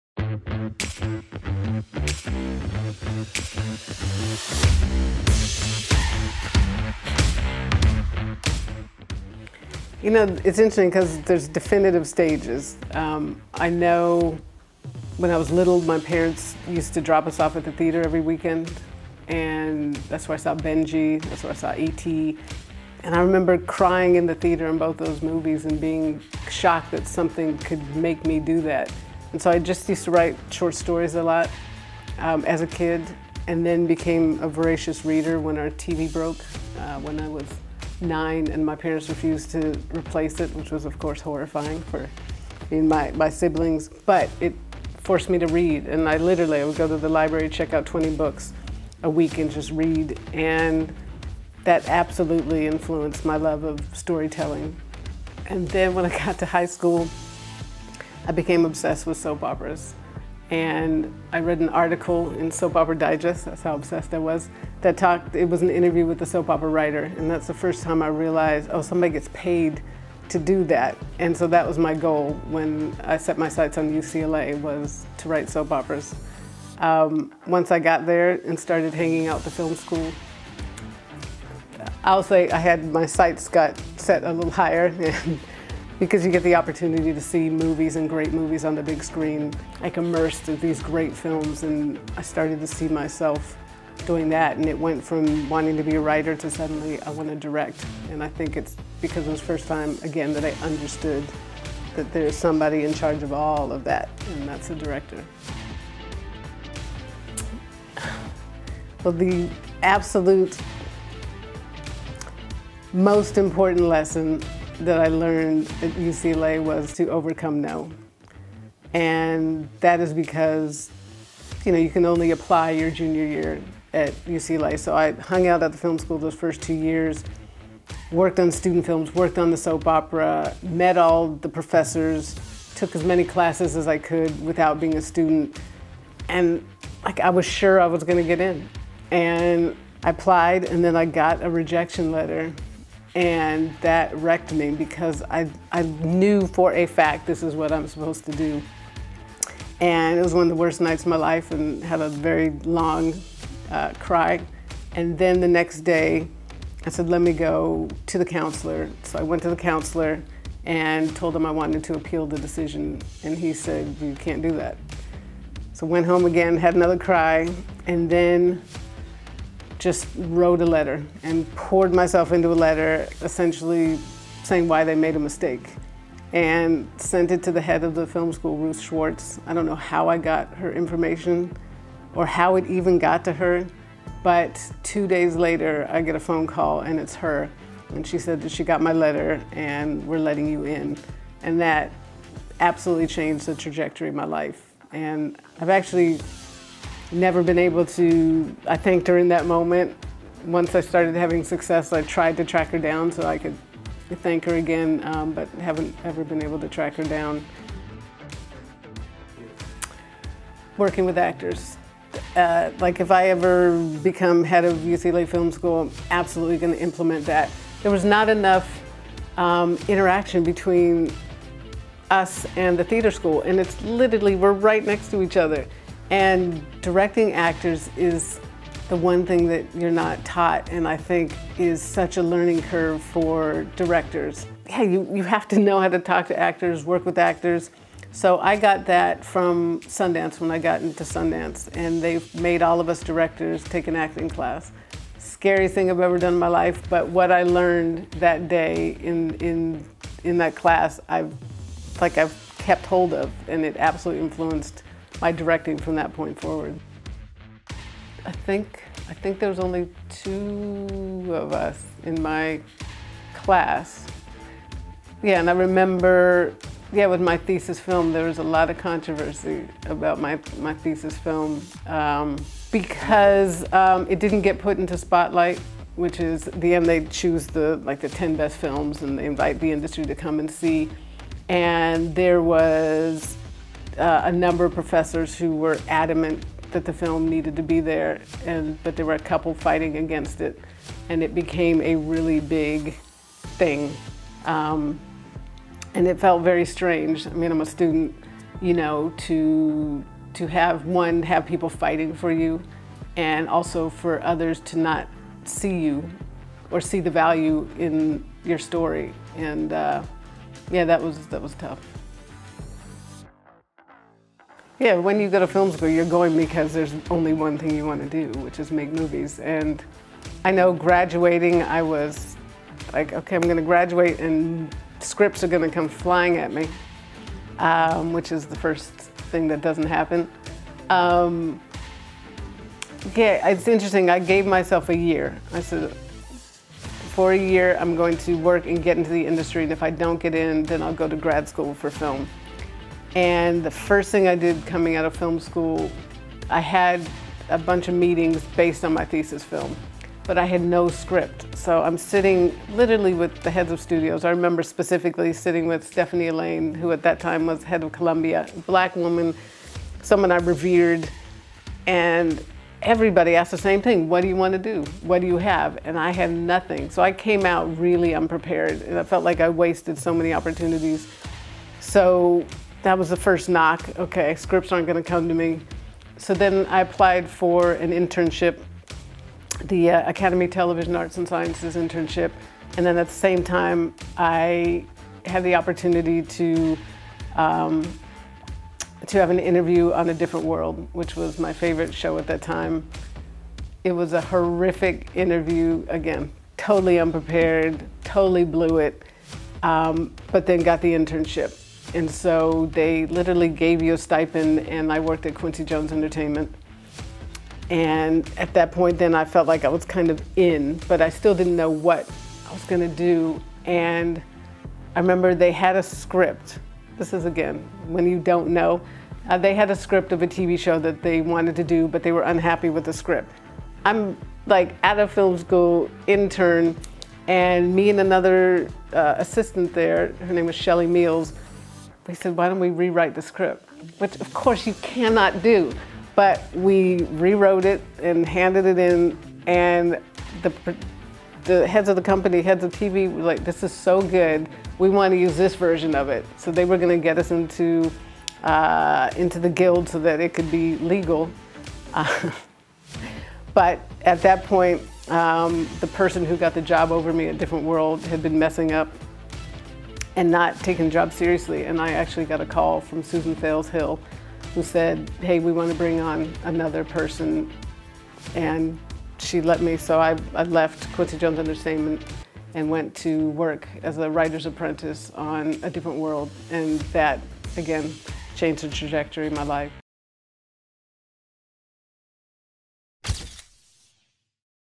Gina Prince Bythewood on UCLA
The director of "The Woman King" and "The Old Guard" sits down for an in-depth look at her career.